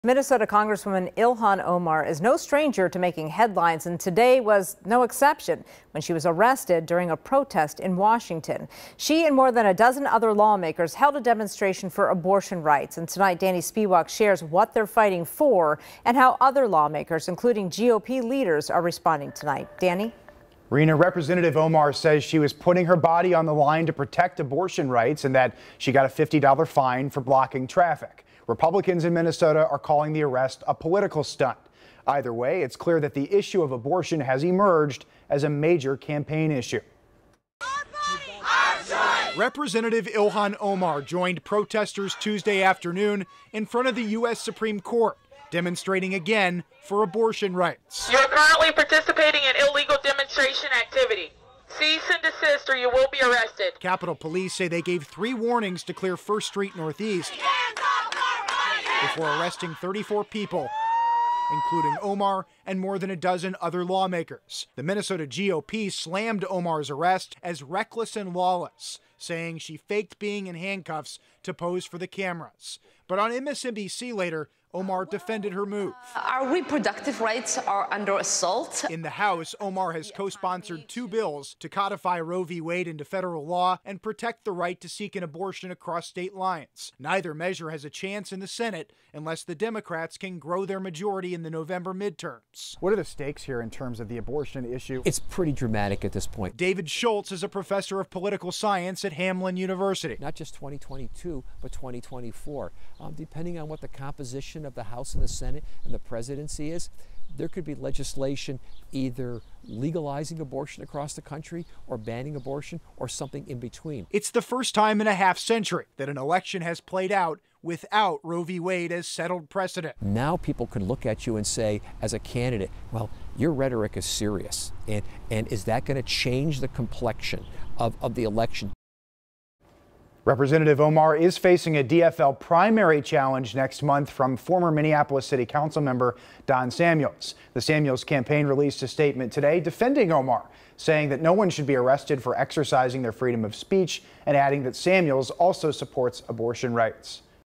Rep. Ilhan Omar arrested during abortion rights protest at Supreme Court
Rep. Omar was taking part in a planned 'civil disobedience' demonstration when she was arrested.